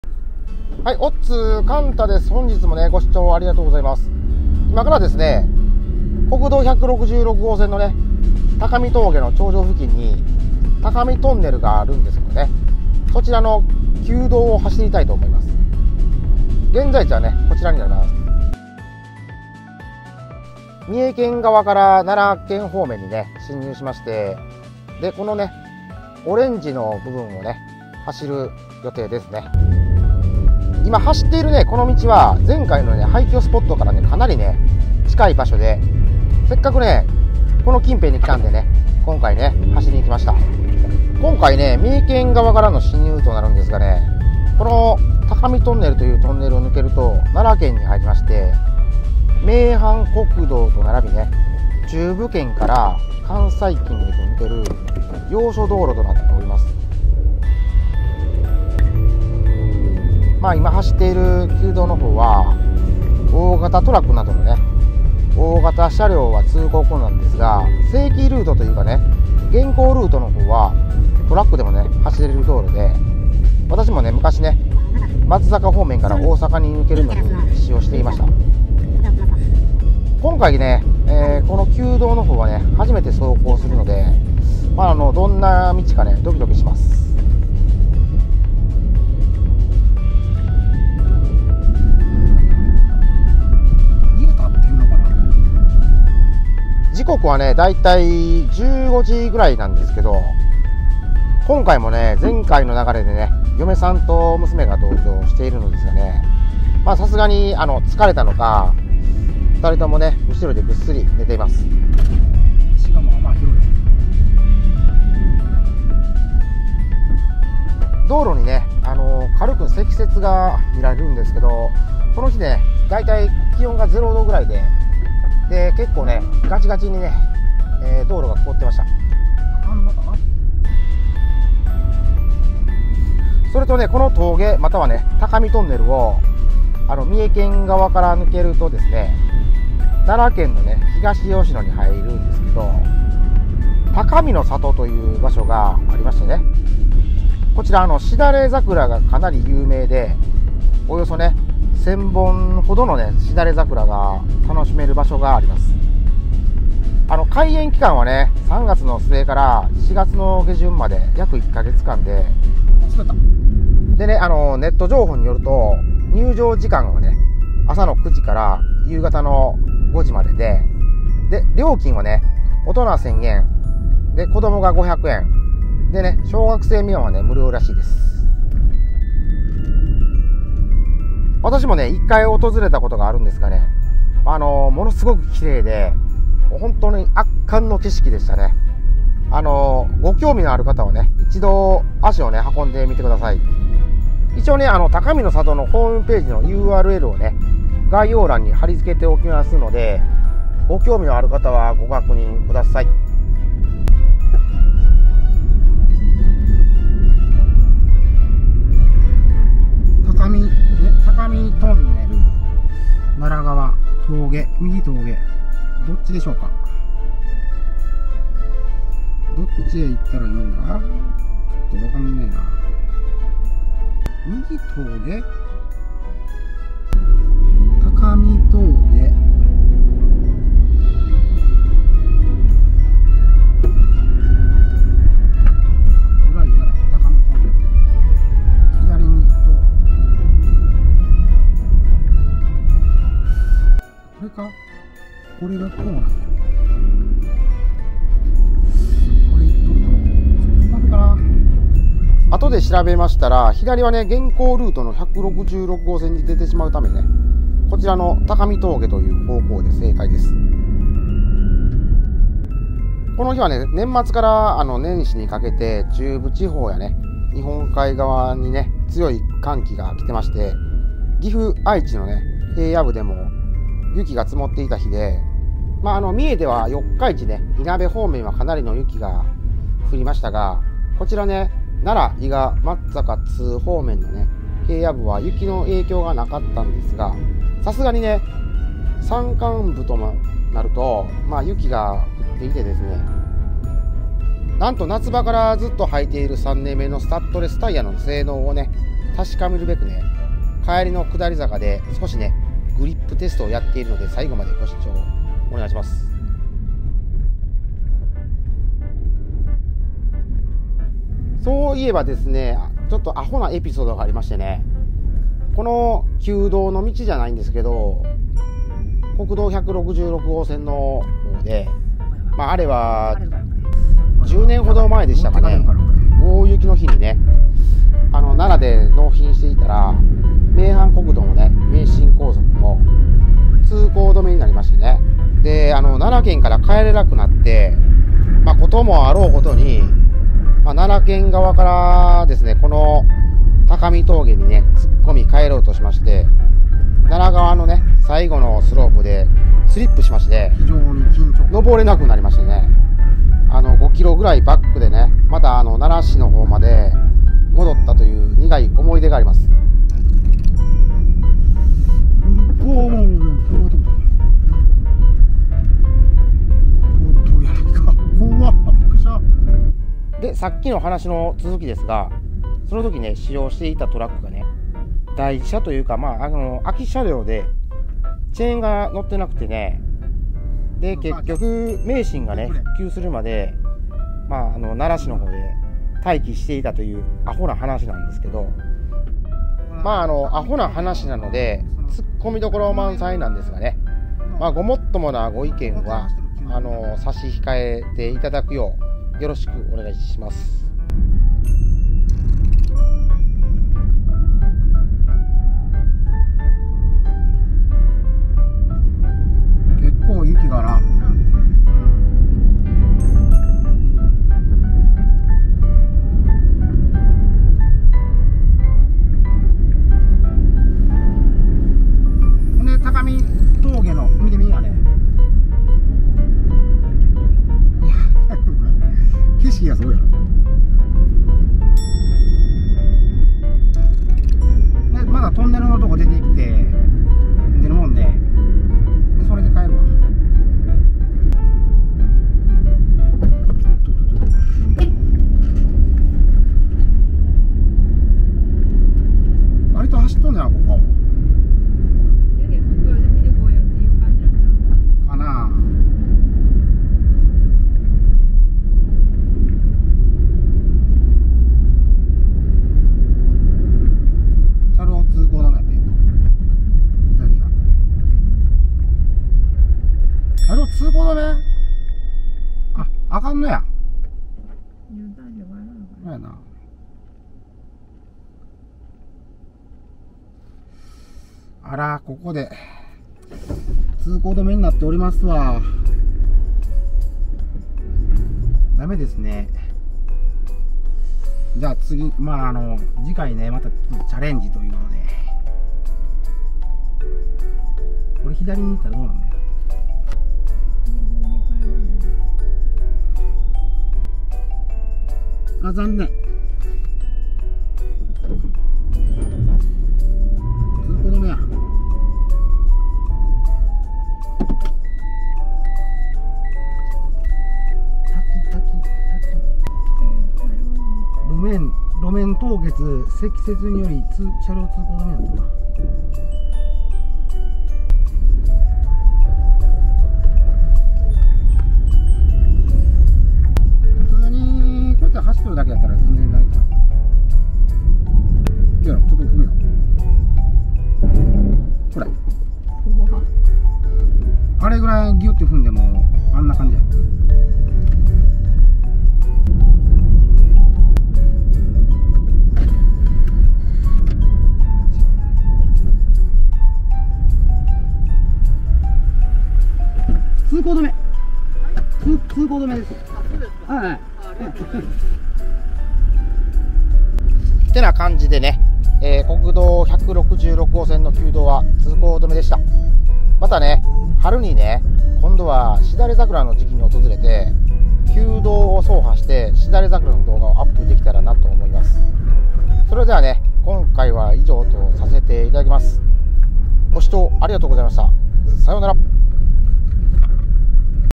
【酷道】家族と高見トンネルの旧国道166号線（高見峠）へ！完走出来るのか⁉ 3期目のスタッドレスで挑む！︎
今回は、元旦に家族で蓮ダム周辺の廃墟探索のついでに、国道166号線（高見峠）の旧道を走ってみました！
この日は寒波が中部地方を襲っていましたが、奈良、松阪、津、伊賀方面の平野部は雪の影響は御座いませんでしたが、さすがの山間部は、雪の影響が多少あり、山頂付近は雪が降っていたみたいで、道路には積雪がありました！
高見峠の旧道は、走るのが初めてで雪道も今年初の走行となります。
完走する気マンマンで、挑んだのですが【まさか】の結果に！
続きは、動画をご覧頂き、ご確認下さい😀

前回動画
関連動画

【廃墟探索】元旦に家族と三重県松阪市の山奥へ！道に迷って談笑の廃墟捜索　Part 1